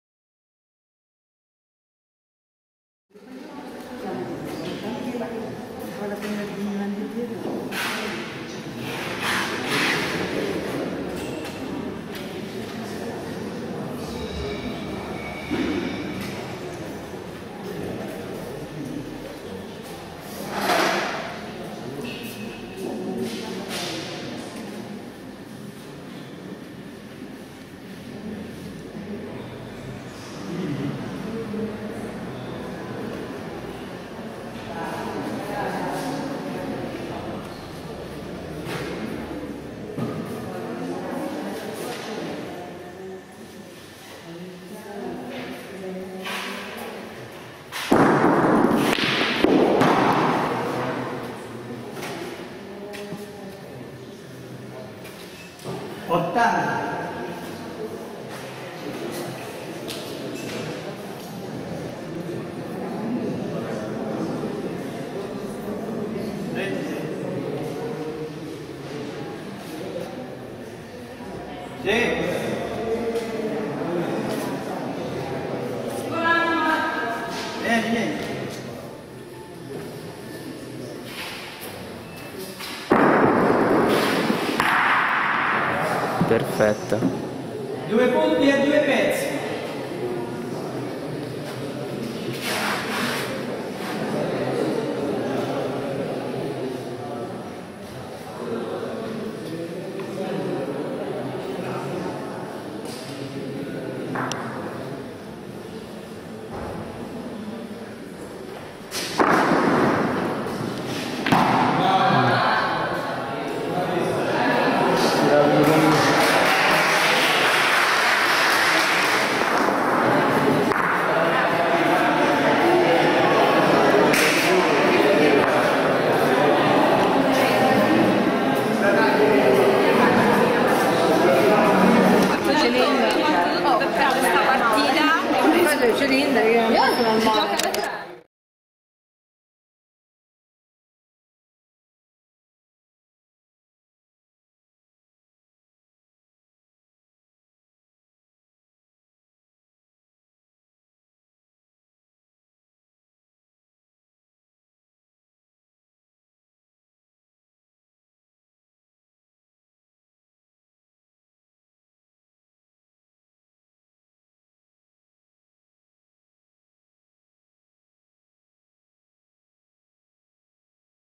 Giocate straordinarie tra Jessica Gelosi e Sefora Corti
Tortoreto Lido 25 Giugno 2012 finale di girone della gara nazione di bocce femminile individuale. Jessica Gelosi (Fontespina) che vinceva 10-4 viene recuperata fino all'11-10 da Sefora Corti (Rinascita). Nell'ultima tornata si alternano giocate straordinarie.